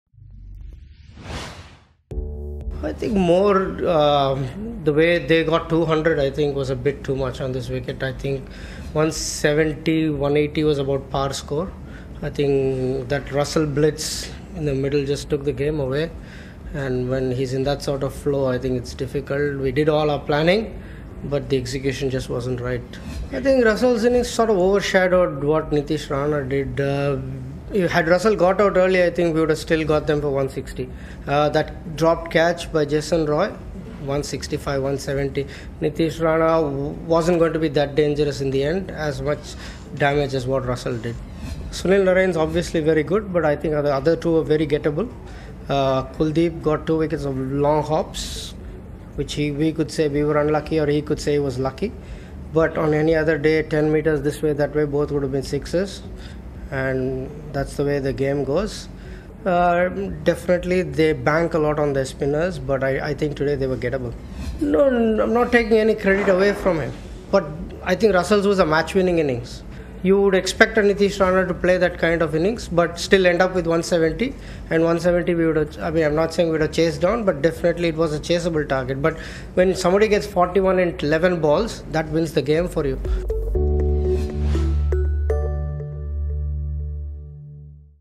Rana played well but Russell's innings was match-winning - Sridharan Sriram
Delhi were completely outplay by KKR in the match at Eden Gardens. DD assistant coach Sriram has a word with the press after the game.